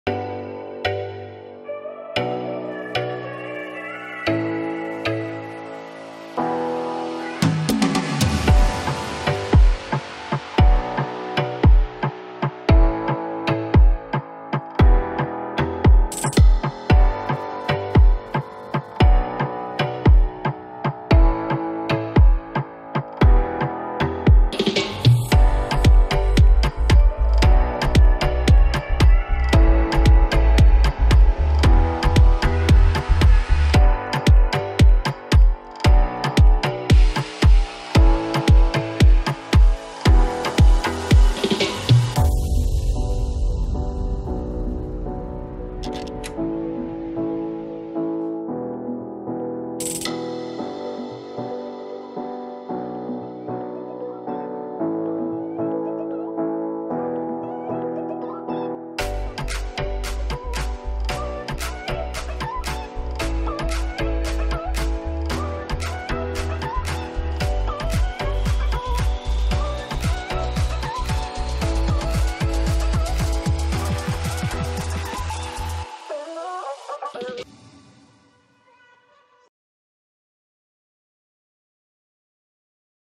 Wie entsteht ein Windpark - VSB Gruppe
Begleiten Sie den Bau eines Windparks von der Anlieferung der Bauteile bis zum fertigen Park.